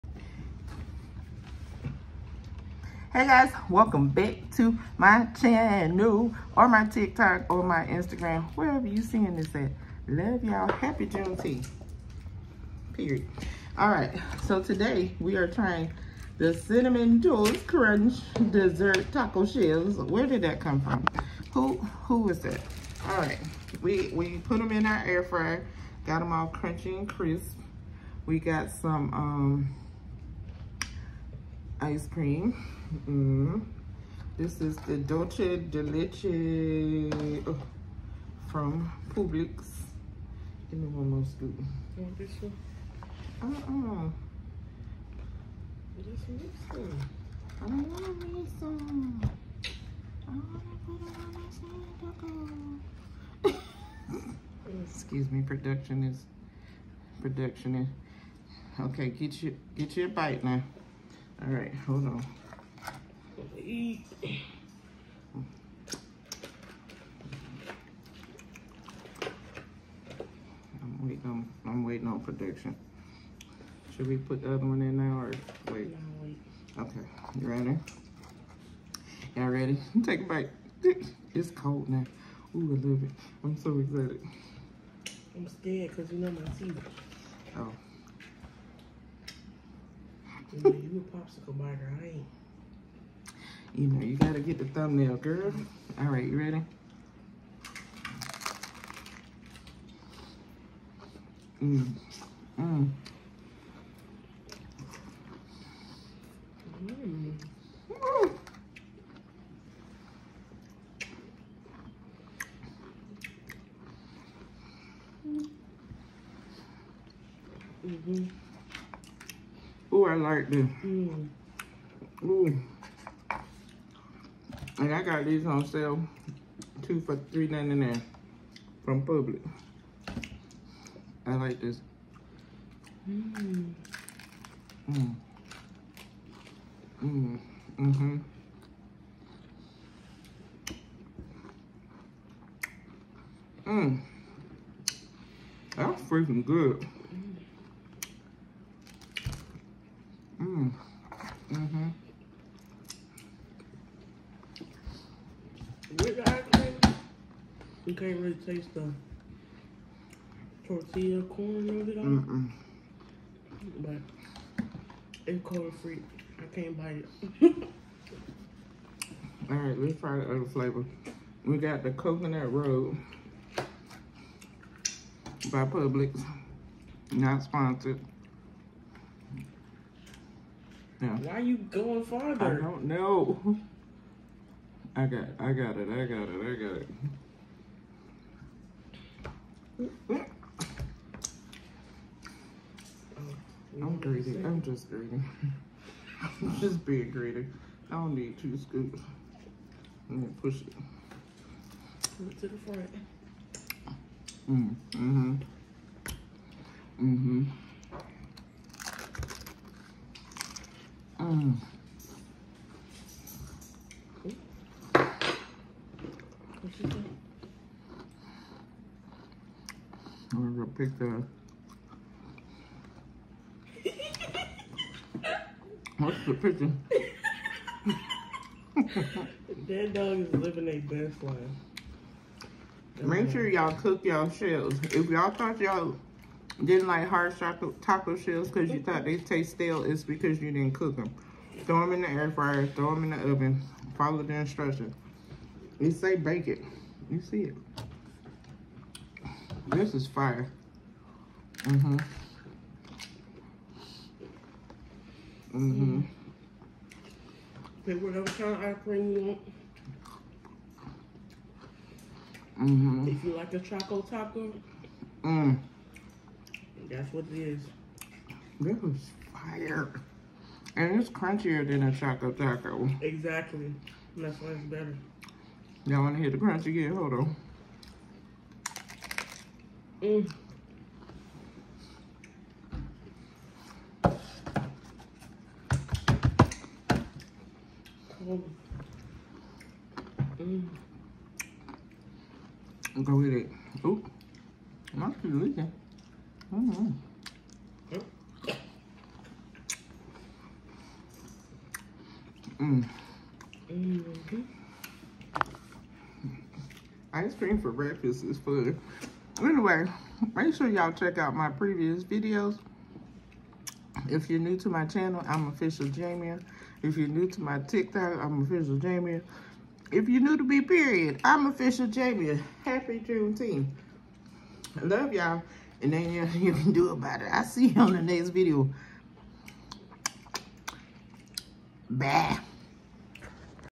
Trying Cinnamon Toast Crunch Dessert Taco Shells!
Heyyyy my Officials!! Happy Juneteenth 🖤💚❤️
Today we are trying Old El Paso Cinnamon Toast Crunch Dessert Taco Shells! They were so delicious!! I definitely recommend them!